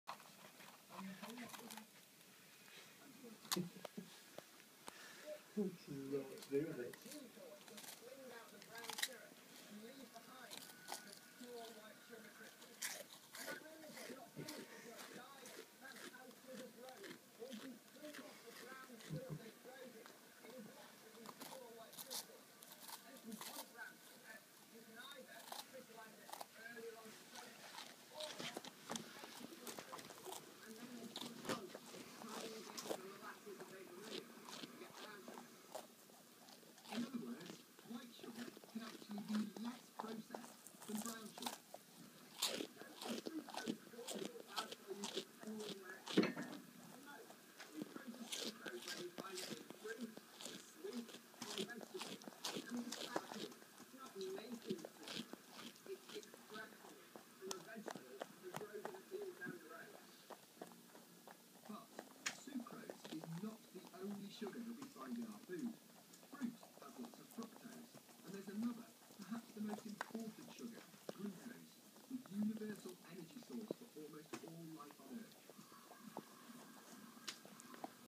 Golden retriever eating shredded wheat
Amber usually has her shredded wheat crumbled up over her dinner, but we thought she might like it whole for a change.